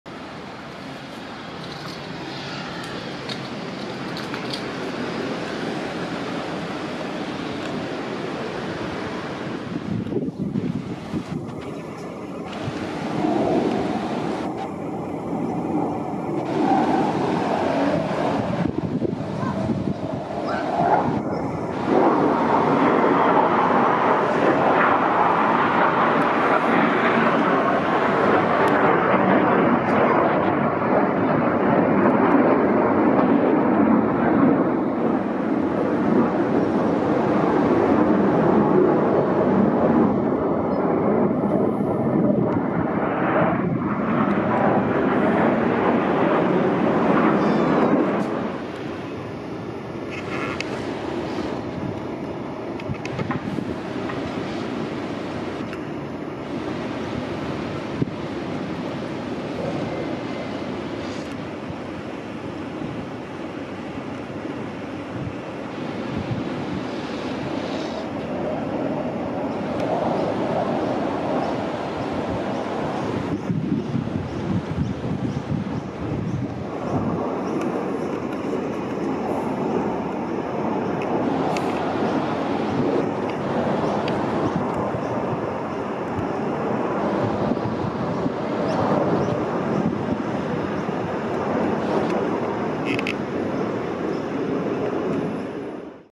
RAAF Growlers over Maroochydore
The RAAF for the past few days and weeks have been doing training up at Townsville.
Today I was treated to RAAF Growlers from 6SQN RAAF Base Amberley. Flying overhead of Maroochydore.